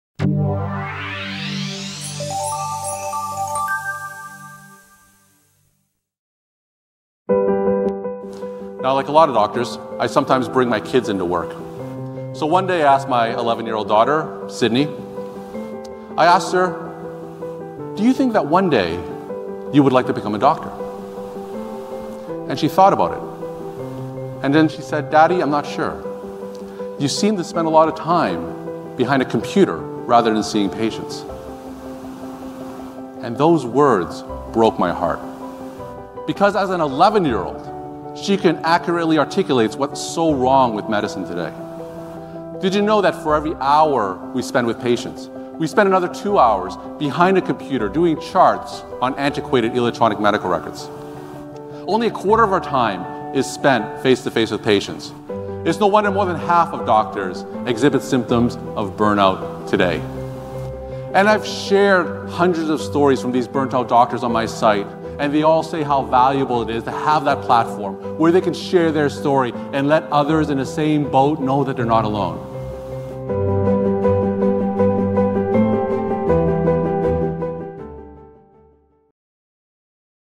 Why We Should Share Stories of Physicians Burnout? - Dr. Kevin Pho
Dr. Kevin Pho is a practicing, board-certified internal medicine physician, a national media commentator, co-author of the book, Establishing, Managing, and Protecting Your Online Reputation: A Social Media Guide for Physicians and Medical Practices and an acclaimed keynote speaker. Dr. Pho is the founder and editor of KevinMD.com, where he also conceives and executes digital strategy, and directs technology infrastructure.